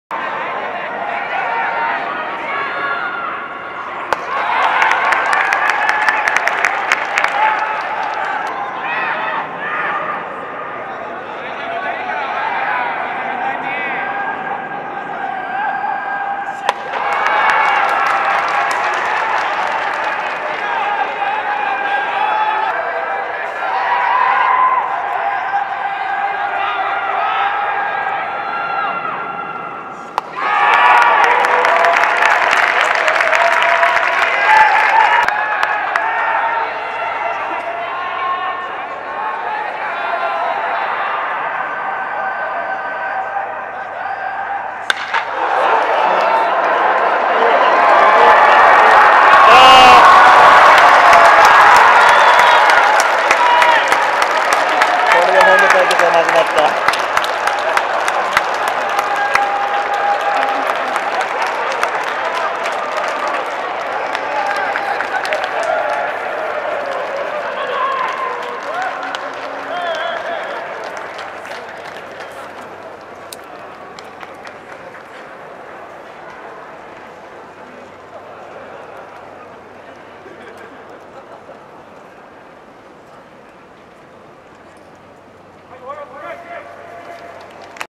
2020/12/01 日本新薬・舩曳 海外野手、サヨナラヒット！
第91回都市対抗野球大会3回戦(準々決勝)・日本新薬－Honda熊本戦は1-1と同点の9回裏、日本新薬は2死2塁で1番舩曳　海外野手(天理高校→法政大)がセンターオーバーのサヨナラヒットを放ちました。